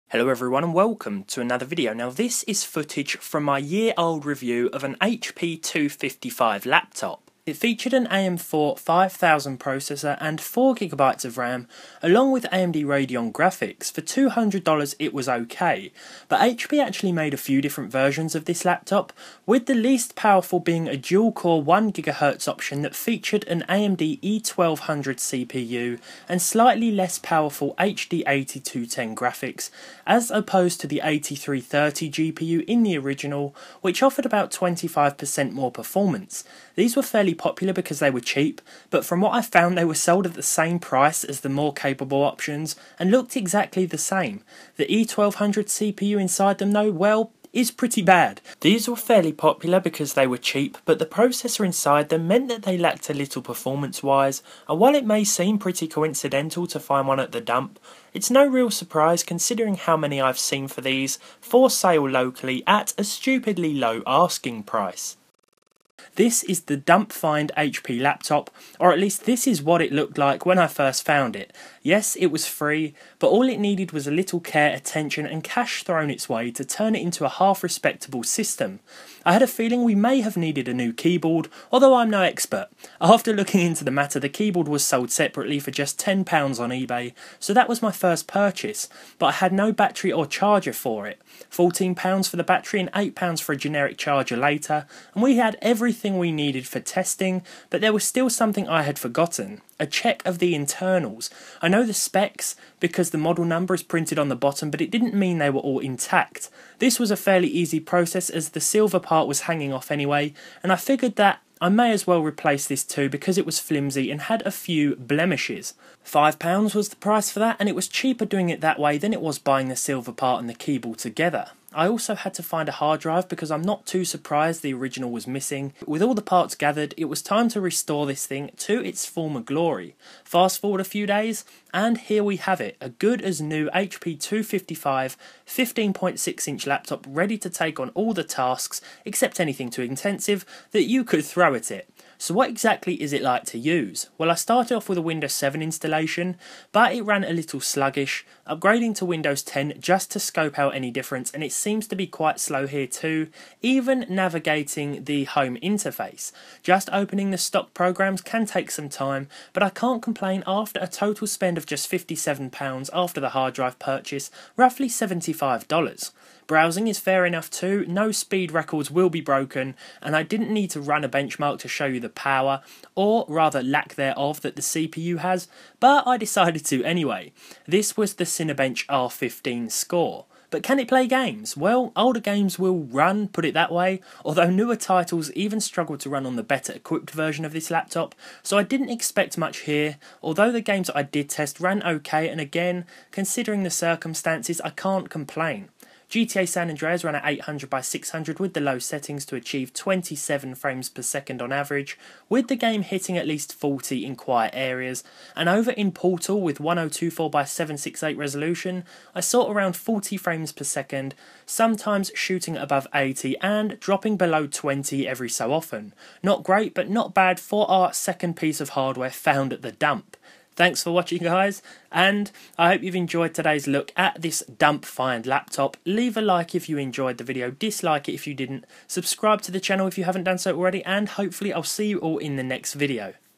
I found and restored a free laptop from the dump
It can be quite handy living near a waste recycling center that also deals with old electronics. Today we're taking a look at an old HP laptop that had definitely seen better days, but with a little care and attention fixed up quite nicely.

Specs:
AMD E1-2100 1GHz Dual Core CPU
4GB DDR3
500GB HDD
HD 8210 GPU